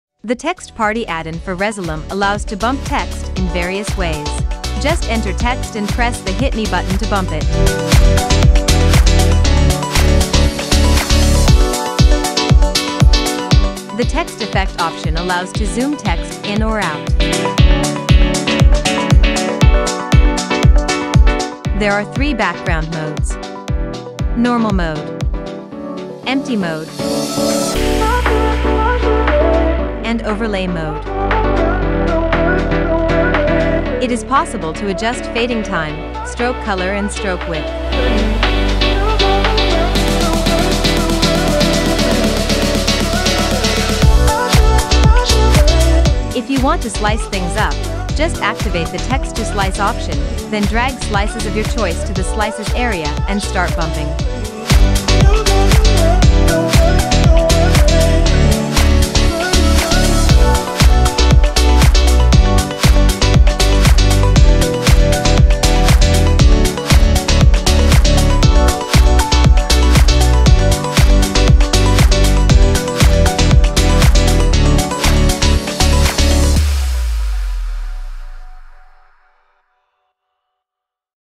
TEXT PARTY for Resolume Arena
The Text-Party addon for resolum allows to bump text in various ways.